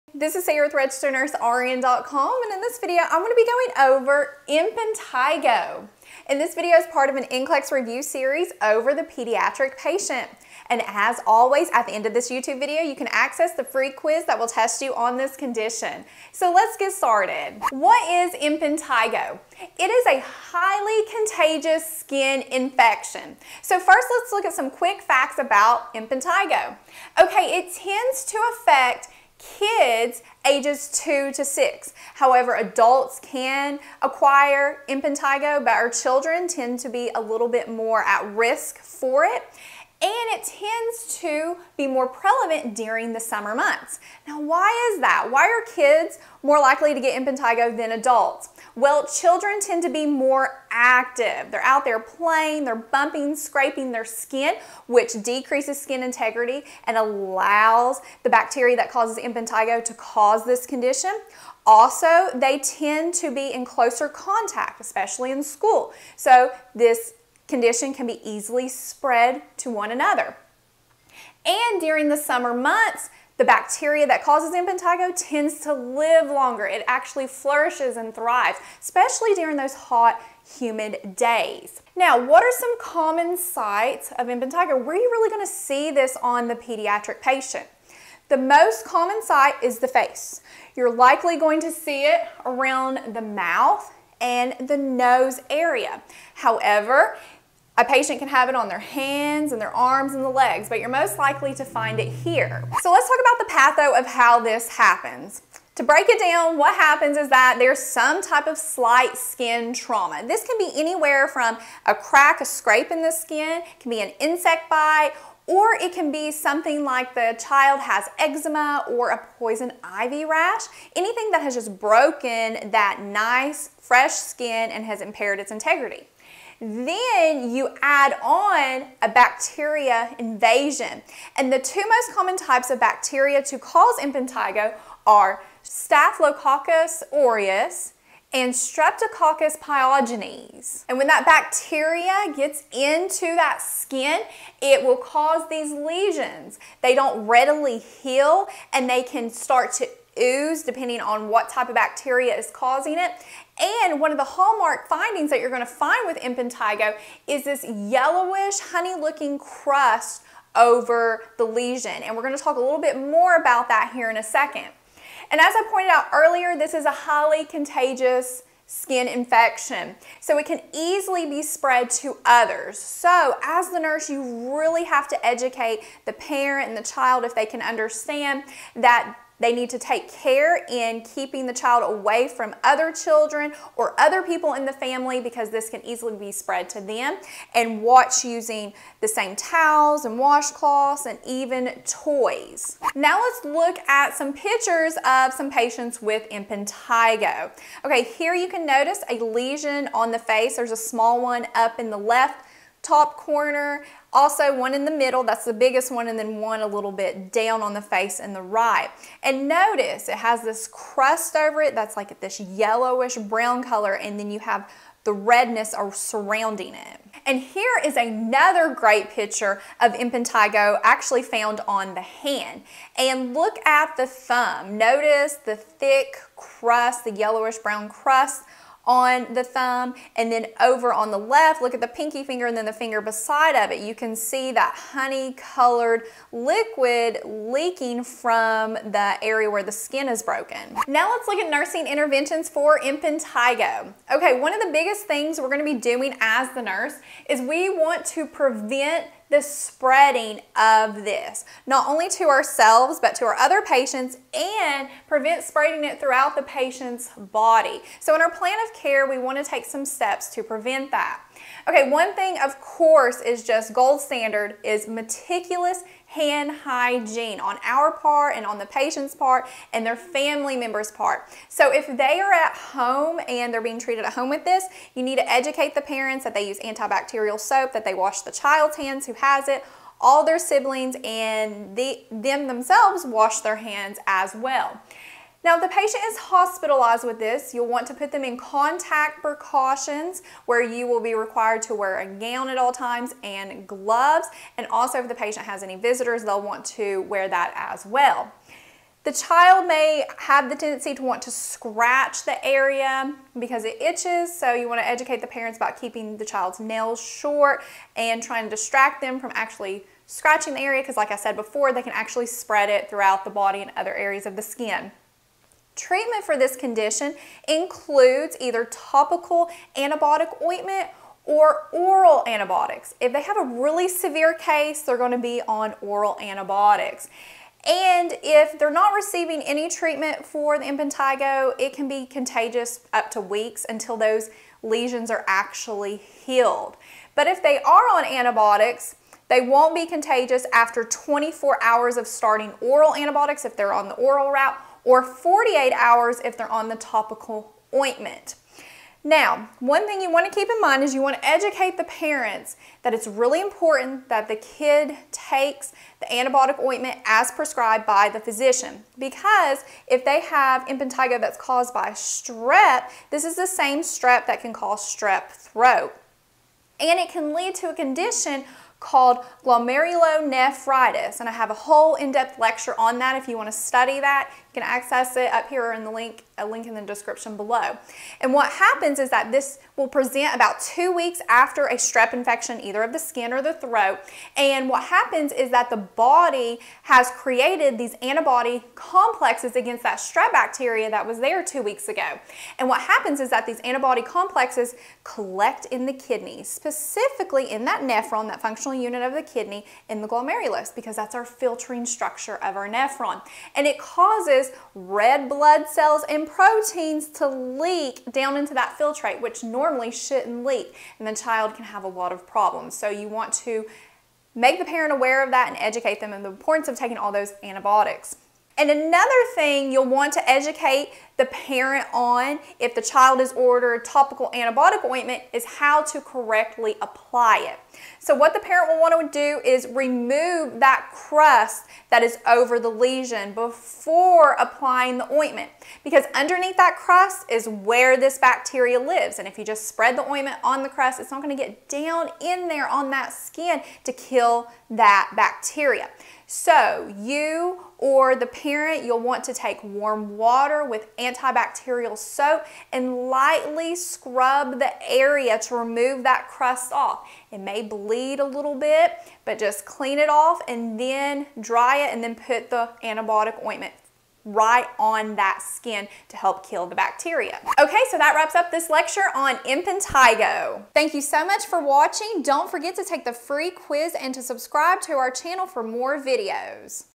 Impetigo Treatment, Symptoms, Nursing Interventions, Pediatrics NCLEX Review
Impetigo pediatric nursing NCLEX lecture on interventions, treatment, symptoms, and causes with pictures.

What is impetigo?

It is a skin infection that is highly CONTAGIOUS!!!

It tends to affect children ages 2-6, but it can affect adults as well. It is most common during the summer months, especially with school-aged kids. The reason why is because kids tend to be more active with play (meaning they bump and scrap their skin more than adults) and are in close proximity to each other to spread the bacteria around.

Impetigo tends to be found on:
-most commonly found the face (mouth or nasal area)
-but it can also be found on the hands, arms, legs

How it happens? A child experiences a slight trauma to an area on the skin (may be a simple cut, crack or scratch or allergic reaction on the skin from contact dermatitis). A bacteria enters this area and causes impetigo.

What bacteria are the most common cause of impetigo? Staphylococcus aureus and Streptococcus pyogenes

Signs and Symptoms of Impetigo:

It will appear as a red itchy blister or bump that will leak and form a honey yellow crust. It can easily spread to other areas of the body and to others. In addition, it doesn't easily heal.

Nursing interventions for impetigo include: prevention of spread the condition (parent and child education), administering treatment (topical or oral antibiotics), monitor for glomerulonephritis etc.